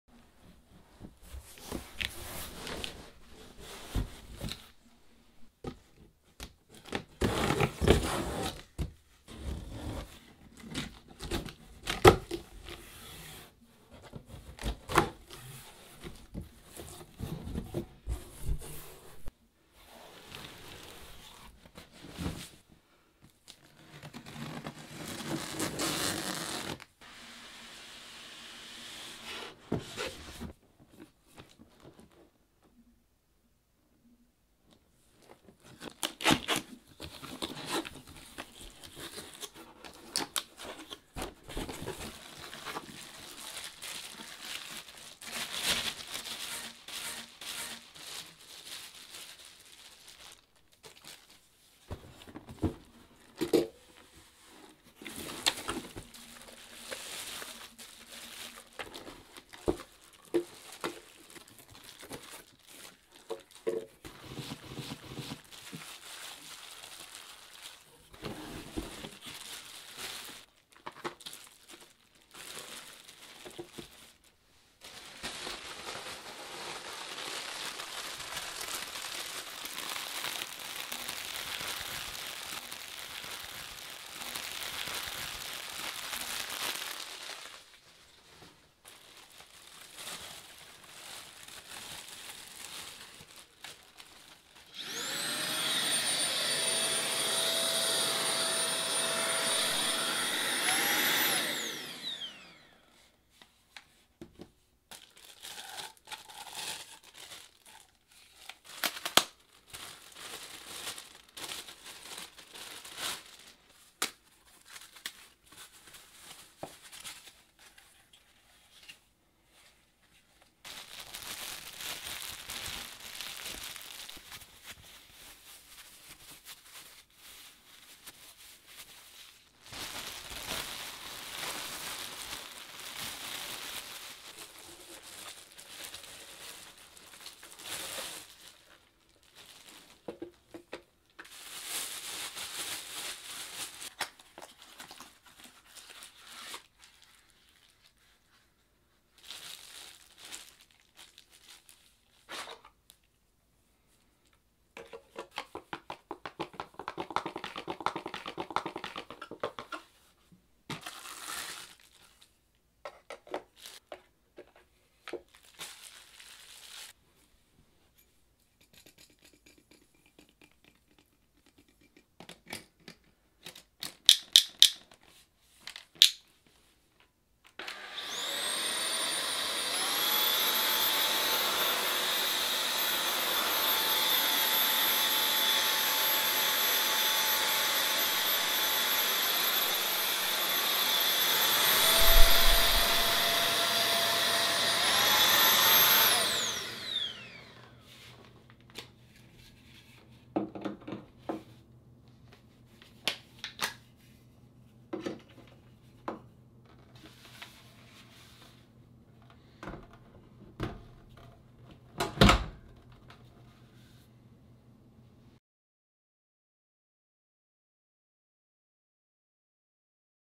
Unboxing the DEENKEE Cordless Vacuum Cleaner
Unboxing the DEENKEE Cordless Vacuum Cleaner. Deenkee sent us their cordless stick and handheld vacuum to try out so here we unbox it and get it ready for a longer test run.

Check out our Amazon storefront for some of our favorite things:

We appreciate your support by visiting our Amazon storefront.